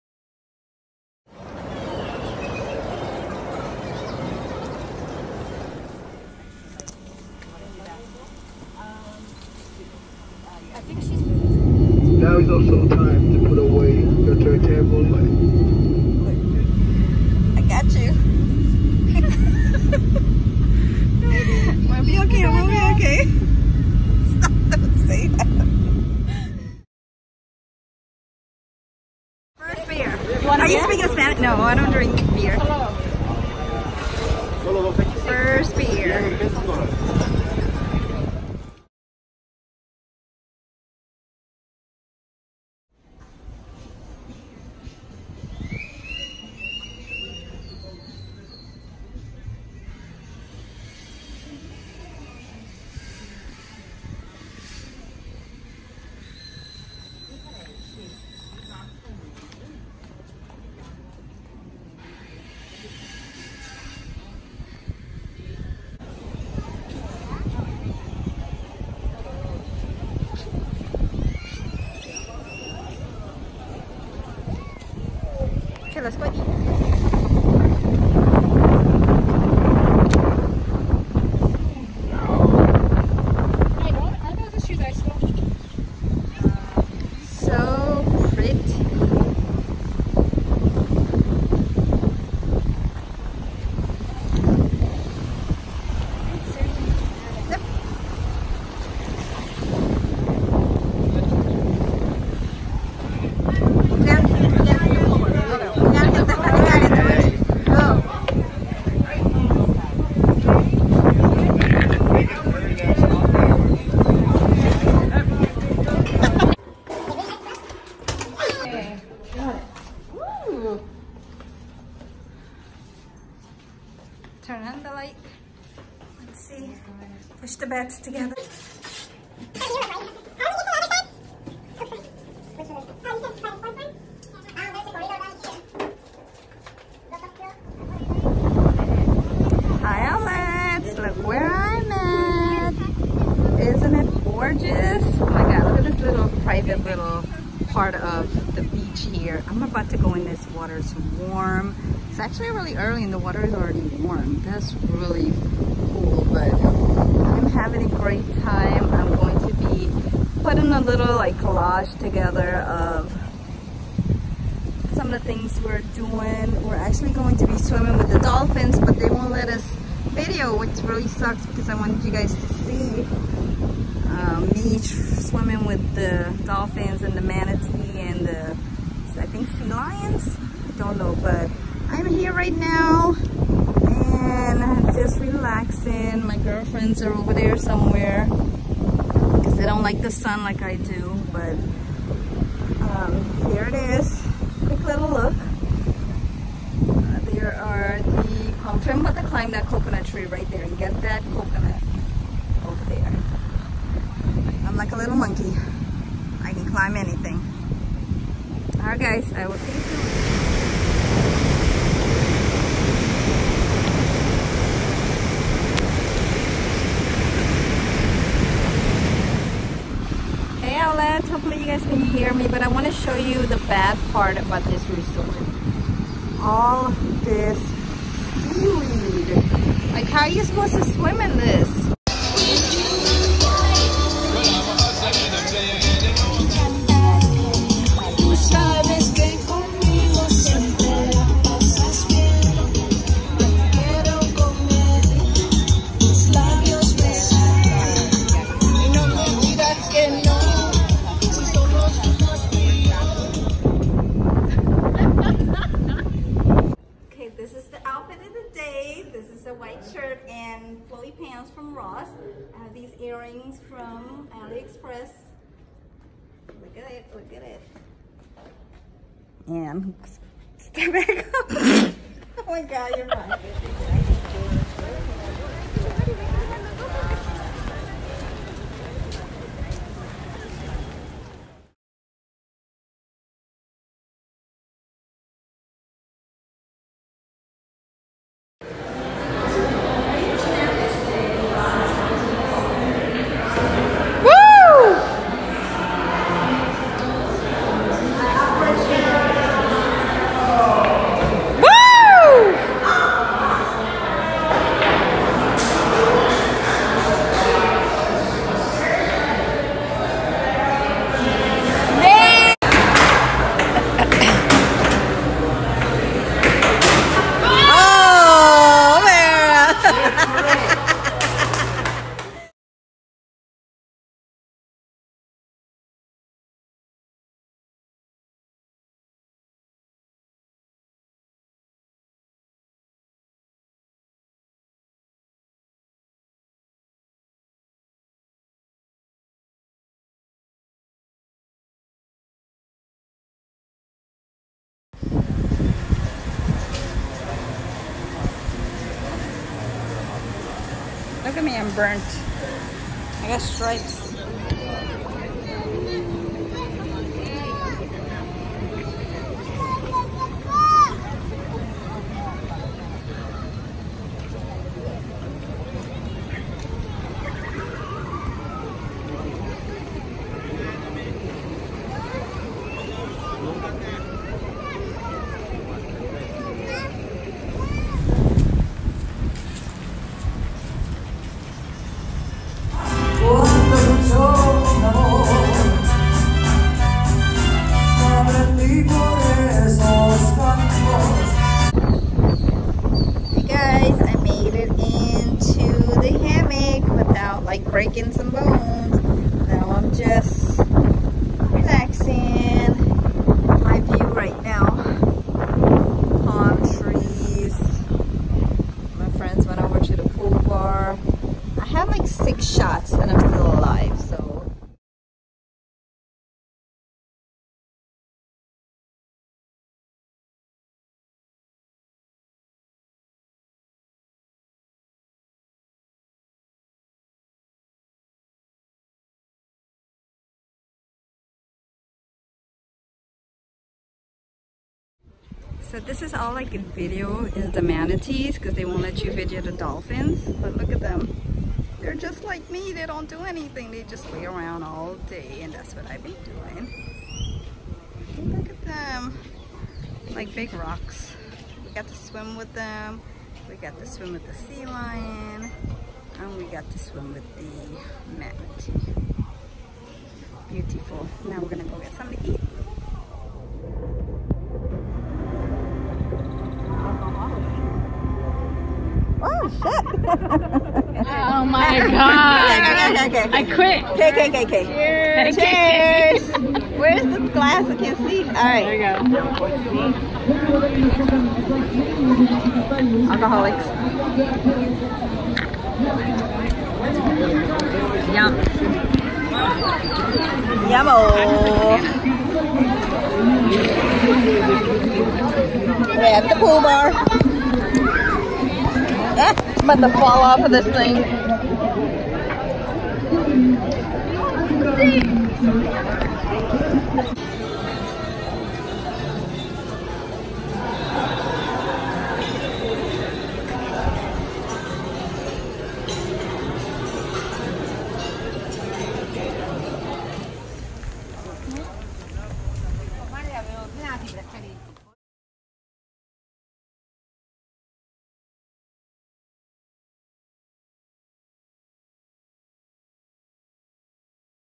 Mini vacation to Mexico 🏝🏝
Hi Owlettes, I thought I would bring you guys along on my little getaway. I wanted to do an outfit of the day but I was drunk most of the time 😂😂 however most of the clothes are from Ross.